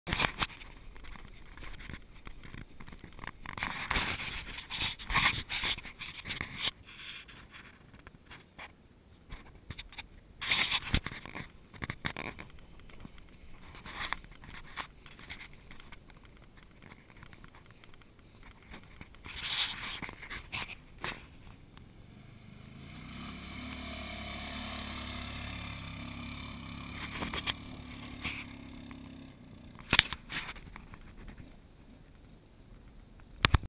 Honda TRX 680 Rincon Pulling Power 2012 HD
Honda rincon pulling cut trees from very wet ground, the tractor around the corner is sunk to the axles.